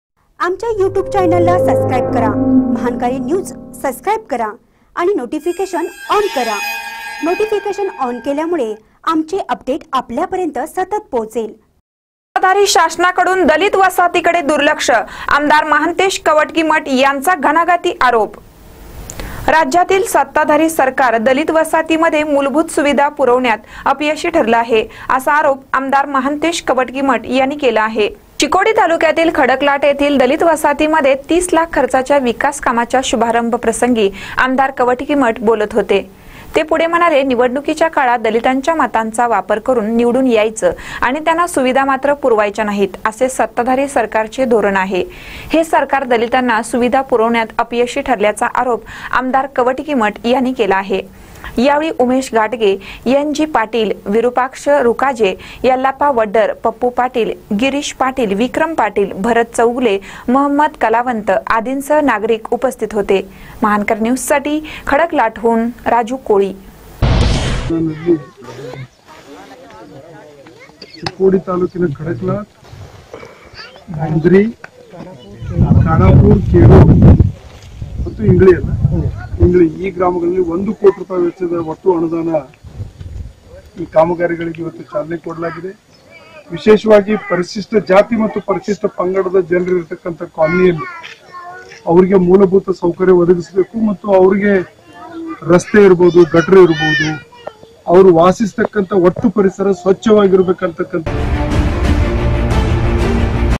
सत्ताधारी शासनाकडून दलित वसाहतींकडे दुर्लक्ष - आमदार महांतेश कवटगीमठ
सत्ताधारी शासनाकडून दलित वसाहतींकडे दुर्लक्ष : आमदार महांतेश कवटगीमठ यांचा घणाघाती आरोप